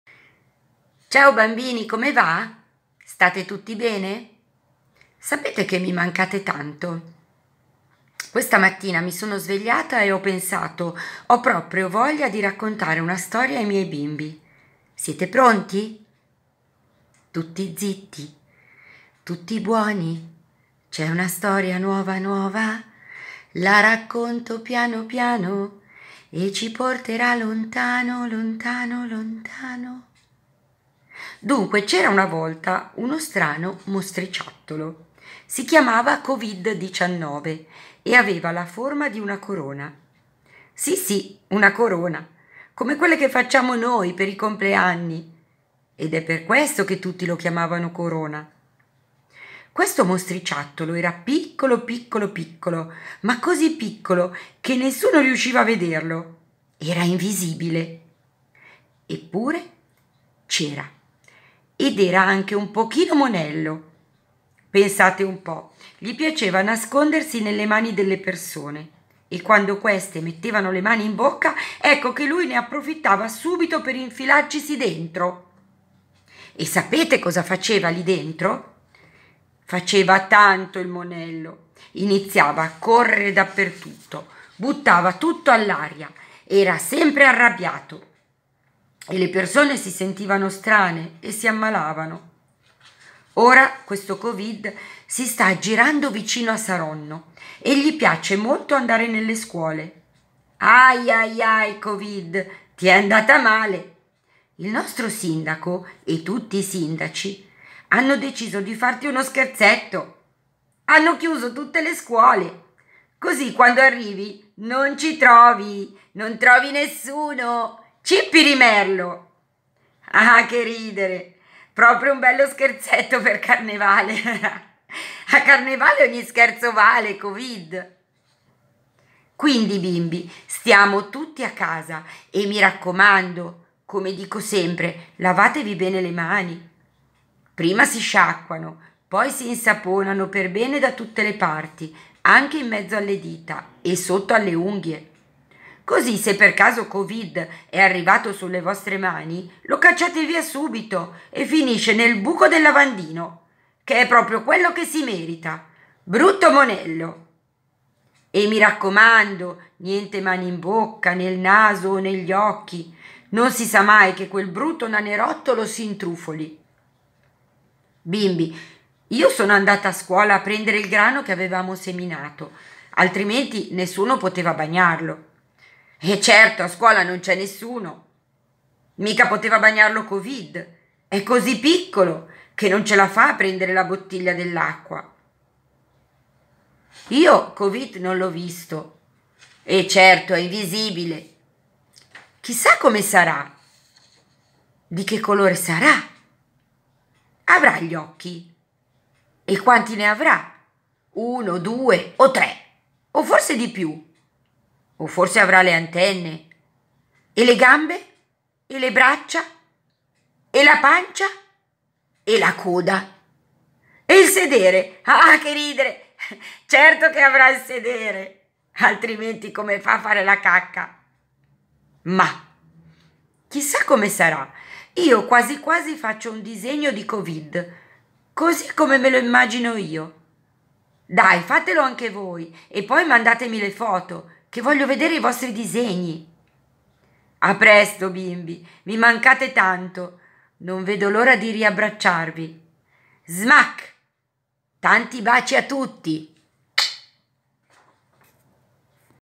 COVID 19 - Uno strano mostriciattolo
Un simpatico racconto per spiegare ai bambini, in maniera semplice e serena,  cosa sta succedendo.